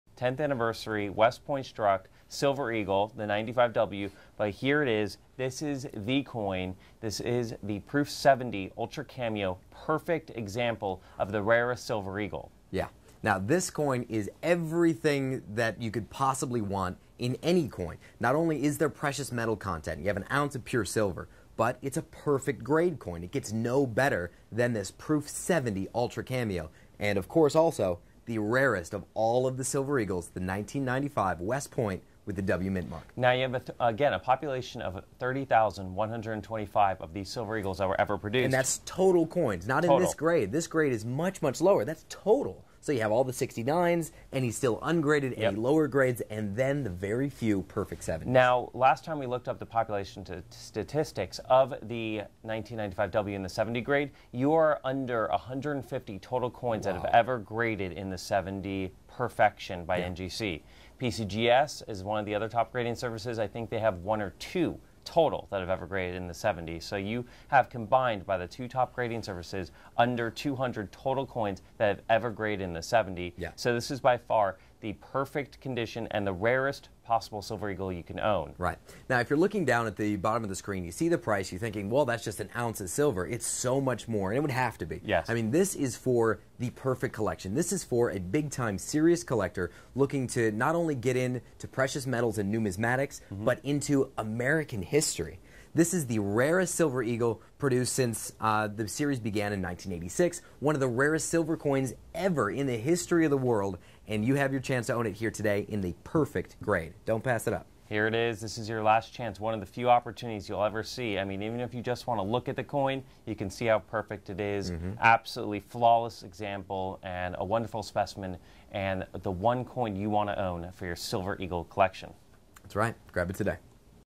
1995-W Proof Silver Eagle
As the Eagle series grows almost daily in popularity, the demand for the 10995-W Silver Eagle grows too. This highly sought-after coin is graded by NGC. Only a few are available for special Eagle collectors and investors, and we have them for you. Add this beautiful and very rare coin to your collection today.